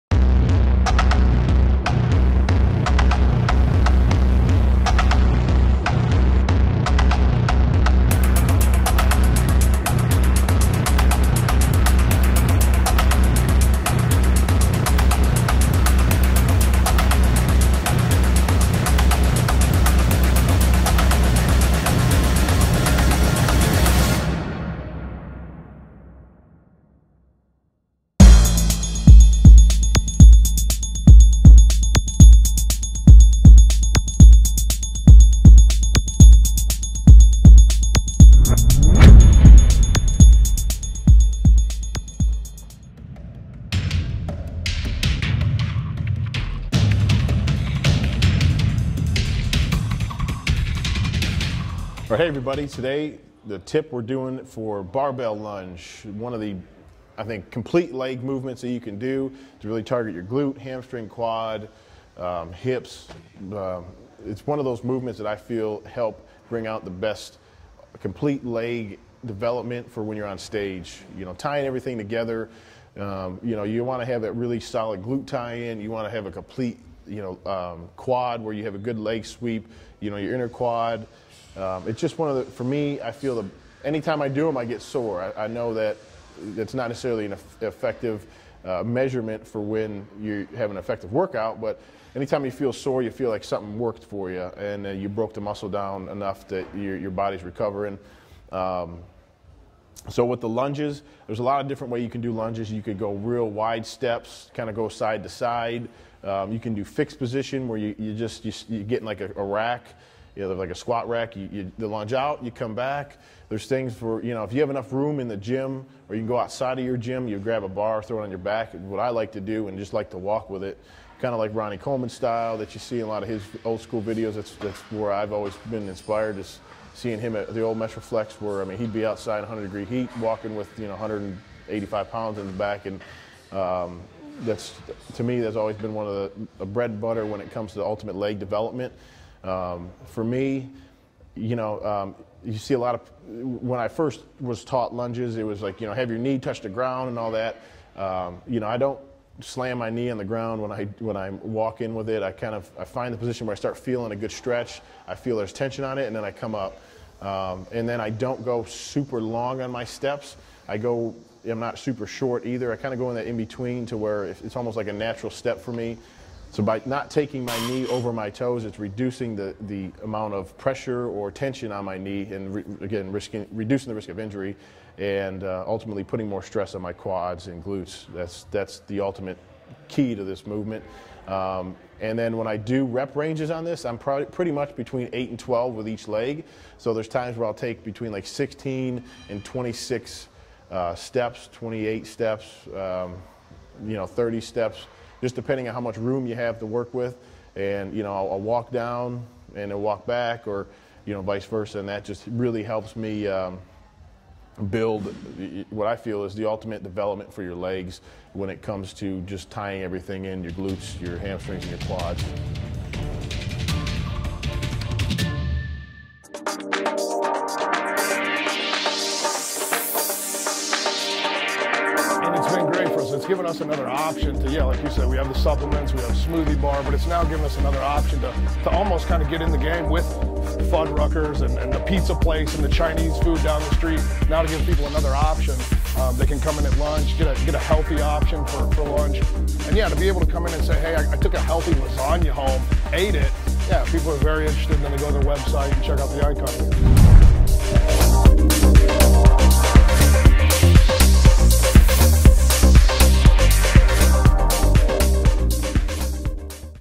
MD 101 - Steve Kuclo on Barbell Lunges
IFBB Pro Bodybuilder Steve Kuclo demonstrates Barbell Lunges.